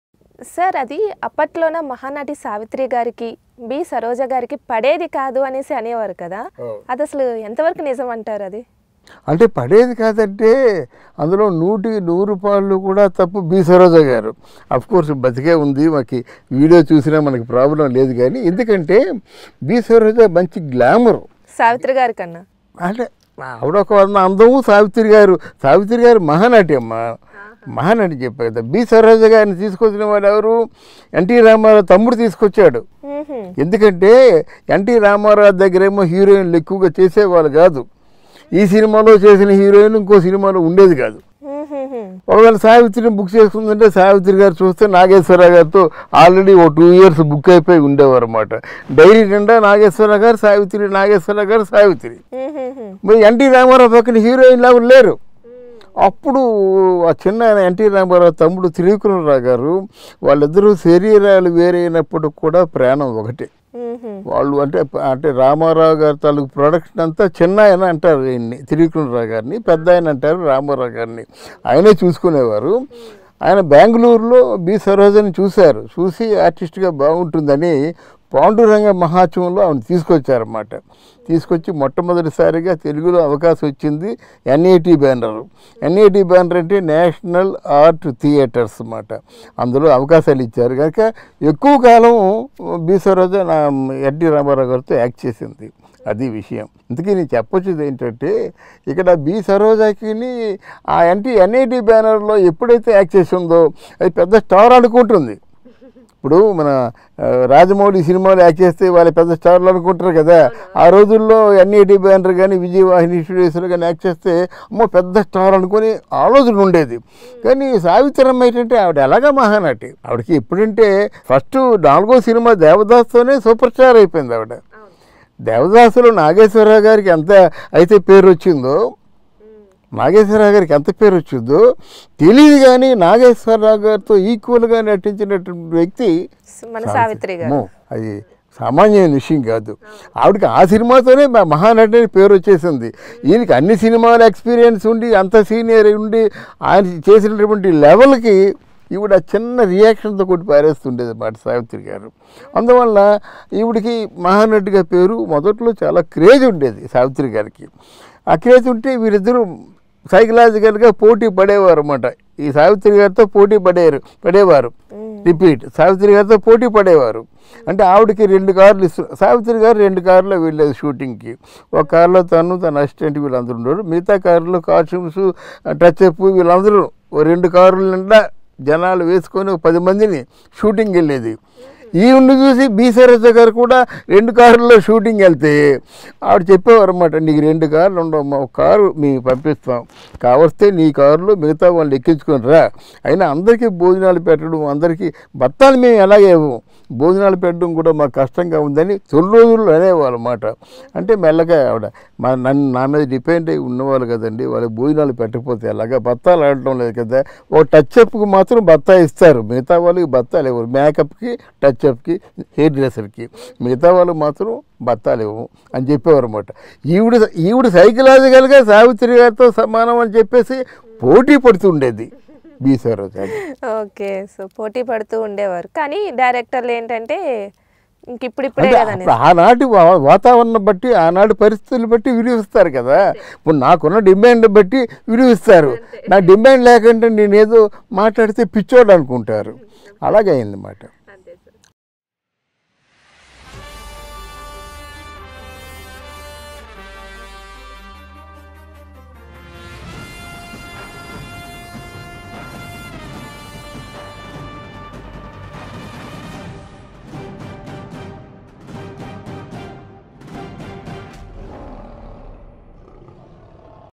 Sr Journalist Imandi Ramarao about Savitri Clashes with Saroja Devi | Savitri Vs B.Saroja
ఆ సెంటిమెంట్ తో చూస్తే సలార్ ప్లాపా? Prabhas Fans Worrying about Salaar Movie Release Date

Leo Politics

Leo Health

Leo Entertainment

Leo Devotional